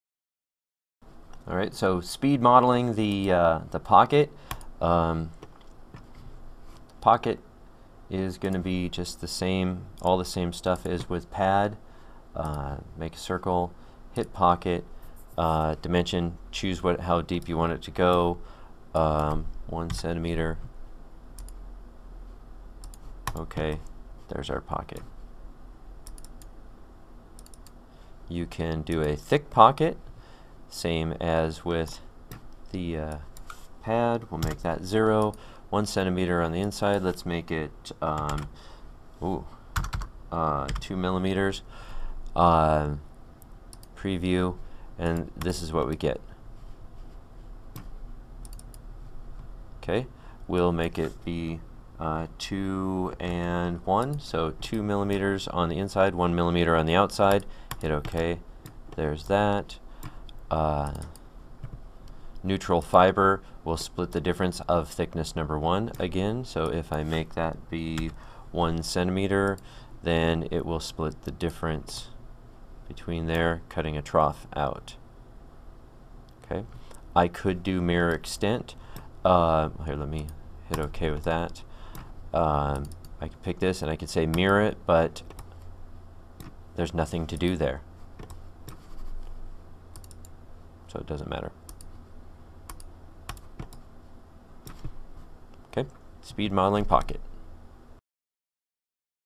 CATIA Speed Modeling - Pocket Tool
This is a short and sweet look at the Pocket tool without as much detail and conversation as in the full training area.